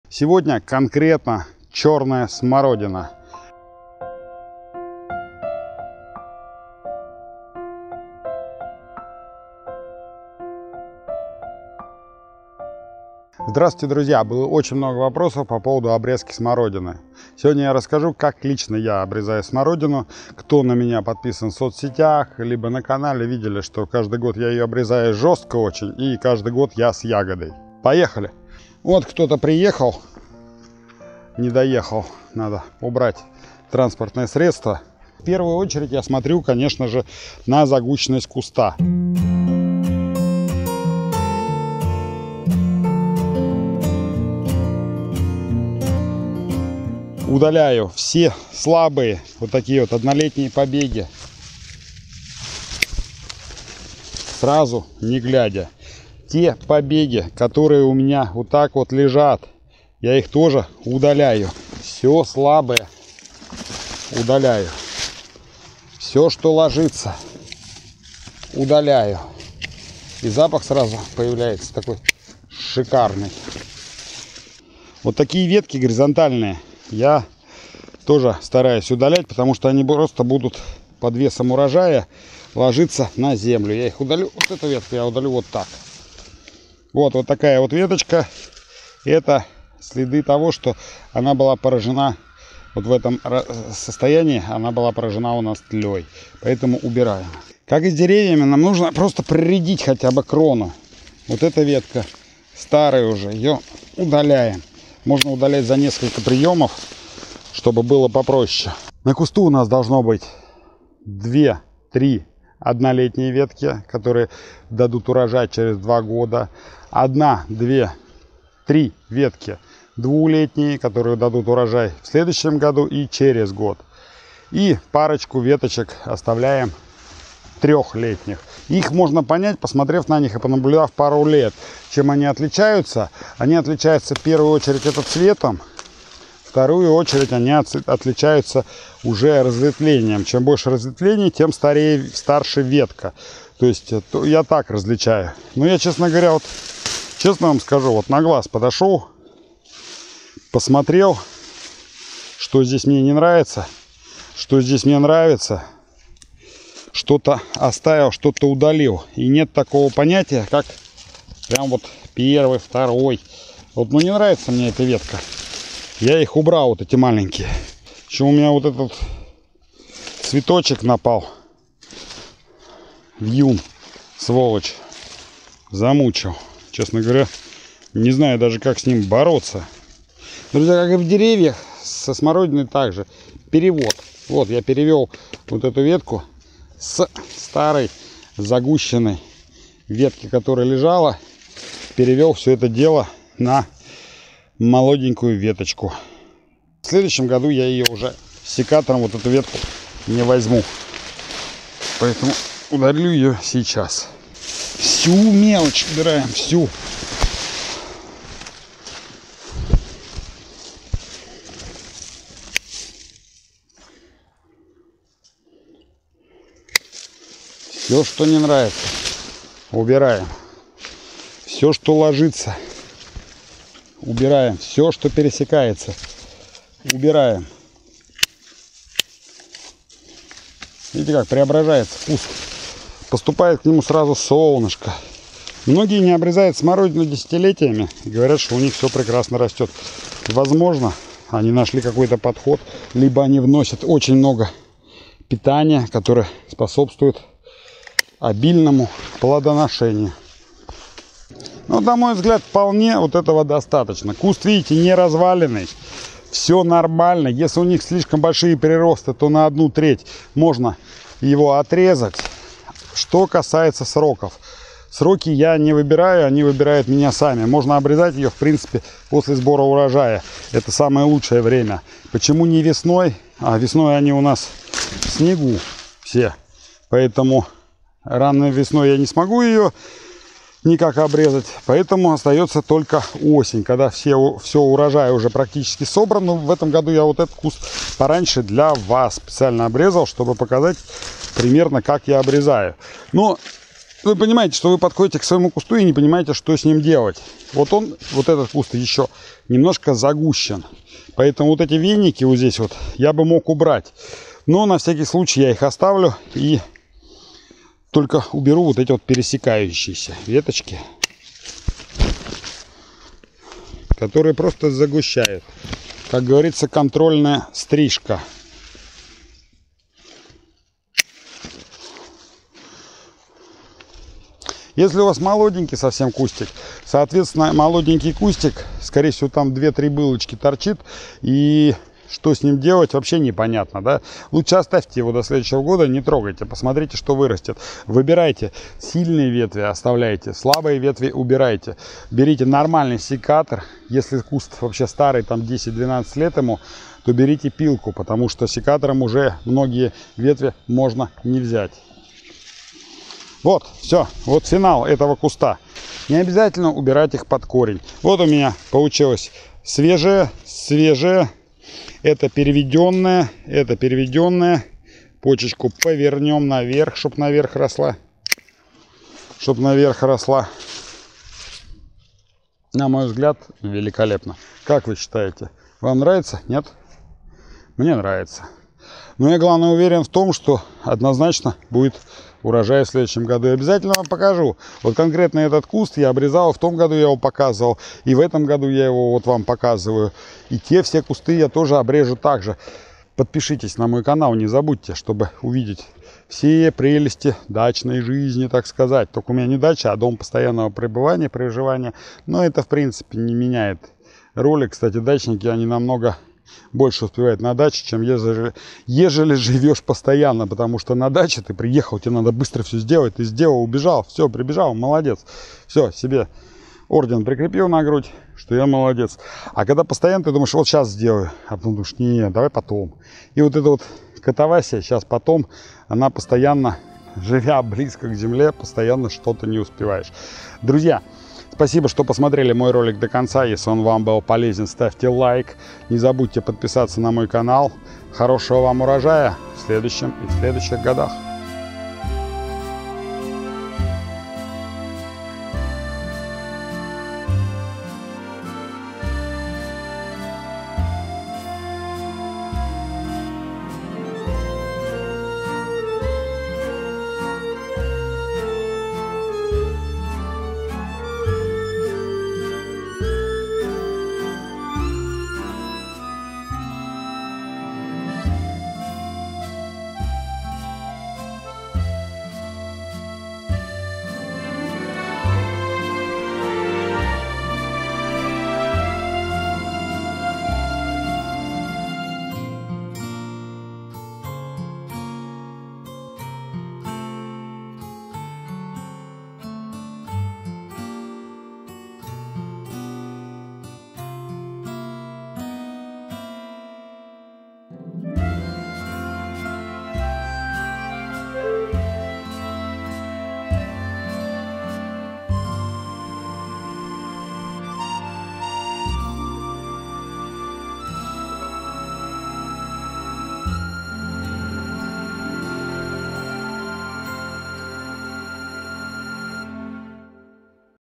если не обрезать черную смородину осенью - УРОЖАЯ НЕ БУДЕТ год спустя или обрезка черной смородины
я очень жестко обрезаю смородину вот например видео обрезки 2021 года этих же кустов смородины